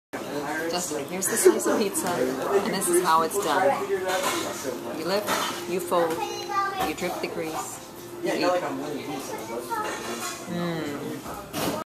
Hot to Eat Pizza
To fold or not to fold your pizza, that is the question.